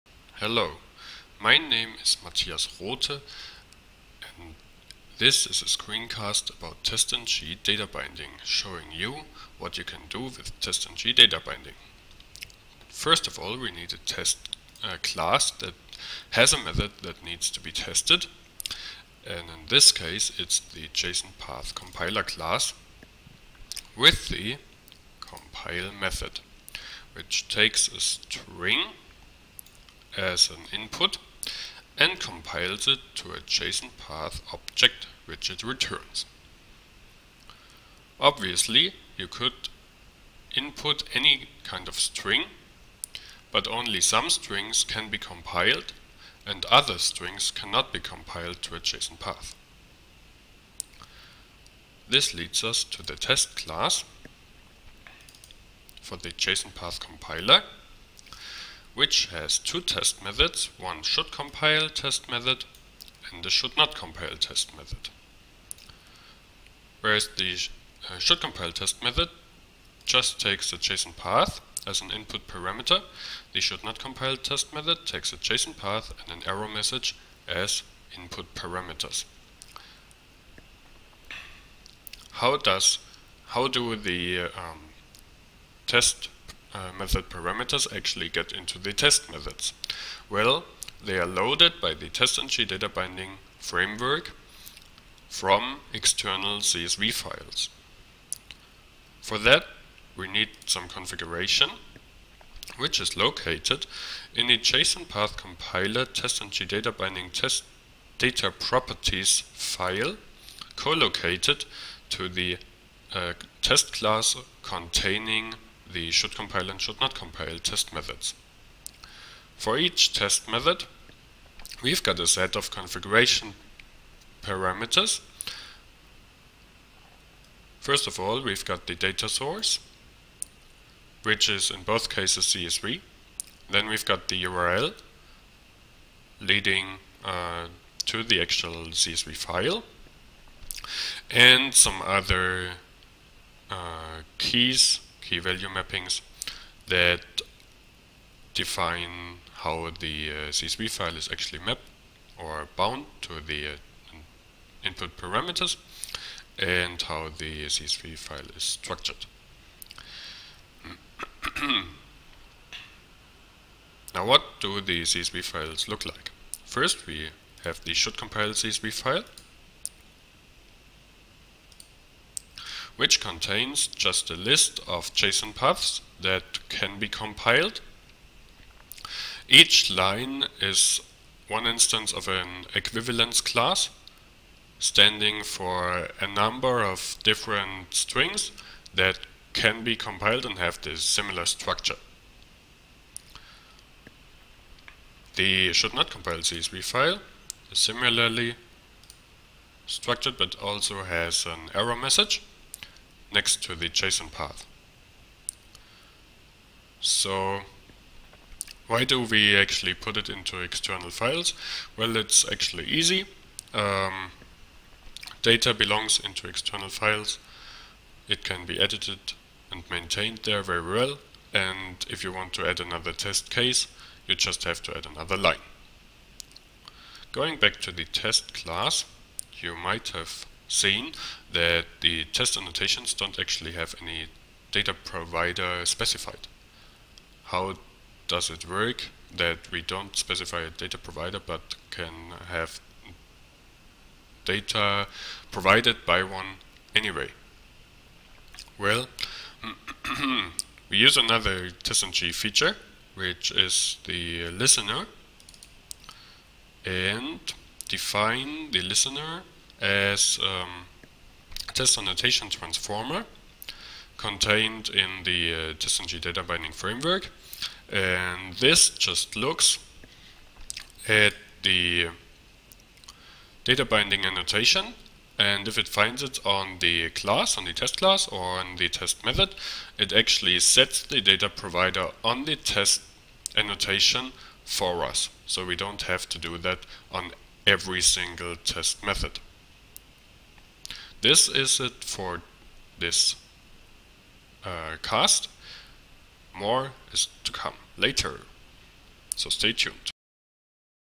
TestNG Data Binding - CSV Example Screencast
This screen cast gives an introduction to the TestNG Data Binding framework showing a simple example of loading test data from a CSV file. The TestNG Data Binding framework presented in this screen cast can be obtained